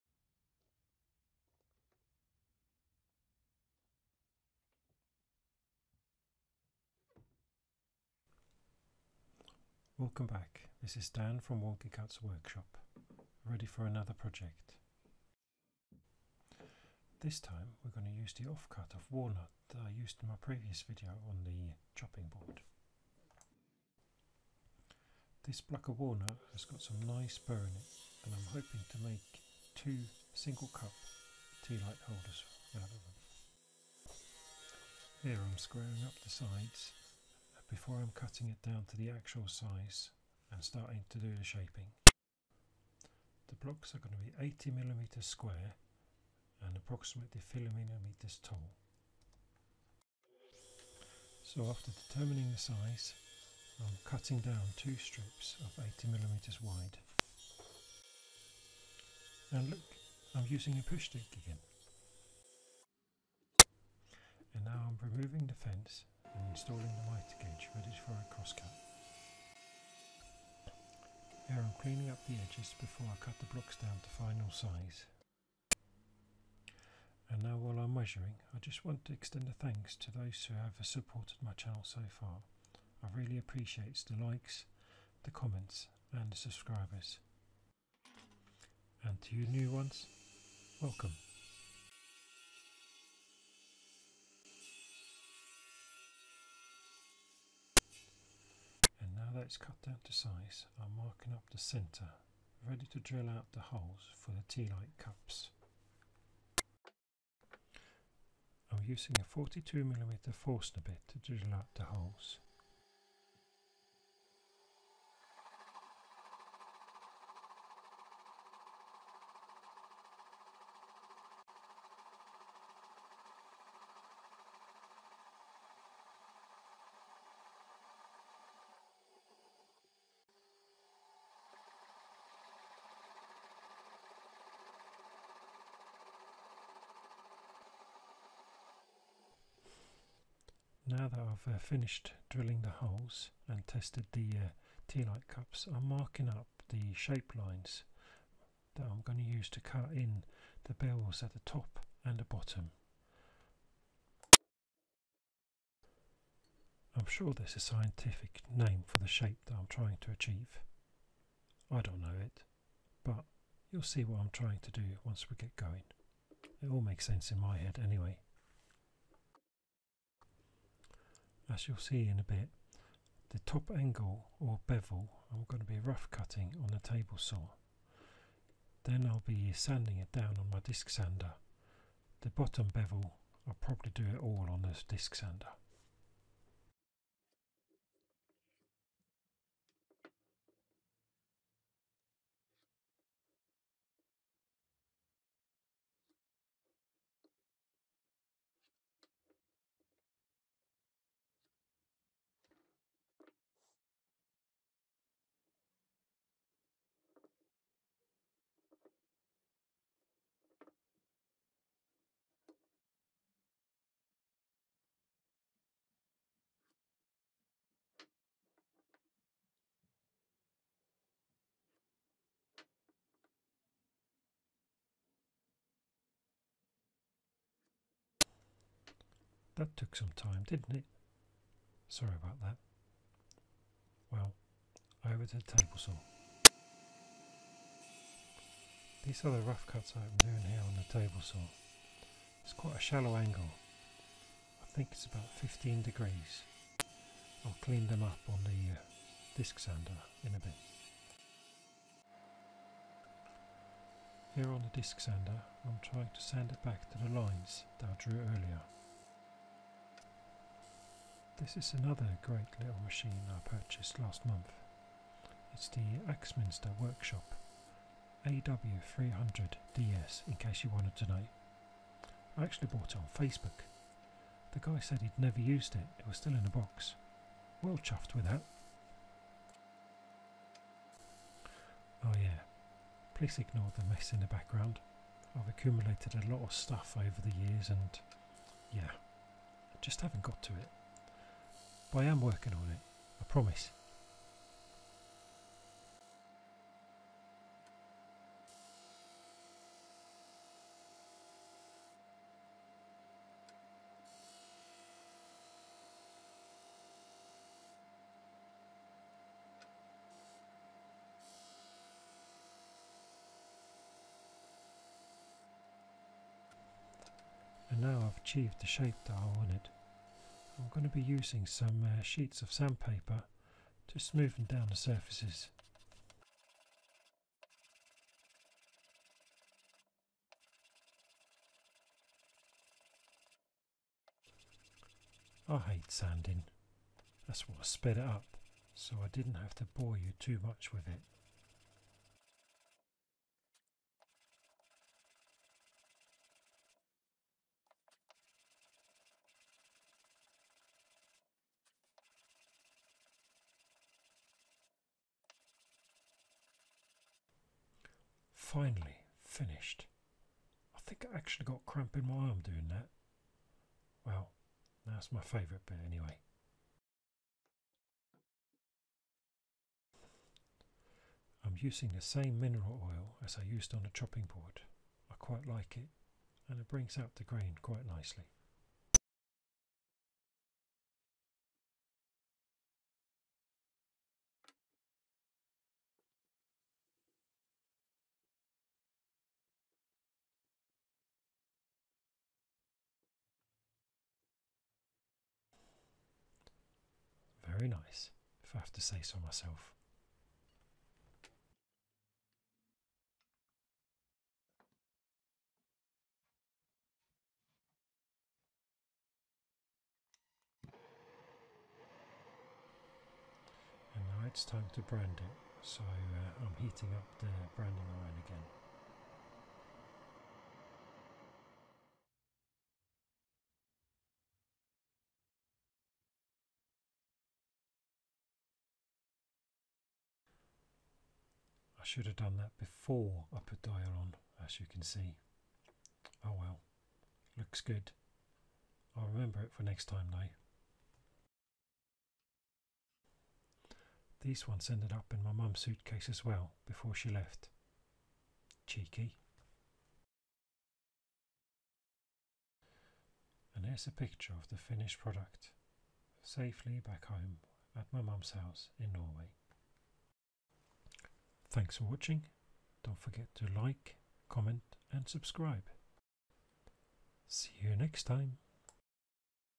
The Offcut Tealight Holders
After having made the chopping board in my previous video, I had some offcuts of black walnut sitting around. It was just too good to be chucked away so I decided to make two tealight holders with metal cup inserts.

It was meant to be an experiment just to see how it turned out but the final result was worthy of a video and my mum ended up taking them with her as well as the chopping board.

I particularly like these because of the grain and burr in the walnut. It really stands out.

The tools I used in this video was Bosch PBD bench top pillar drill, Axminster Workshop AW300DS and the table saw is also an Axminster Craft AC216TS that I have modified to fit in my mobile workbench.